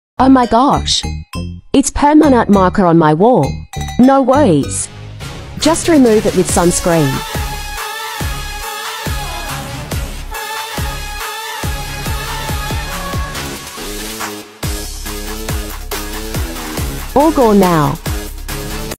How to remove permanent marker on wall or floor
You know how it goes. you turn your head for a few seconds and bam. Permanent makers all over your wall. Your baby picaso is wailing from your death stare, so you call the painter spend way too much money and a few weeks later, it happens again.😩

That's why we're here, peeps.

Try our hacks. Please. It's worth it.😉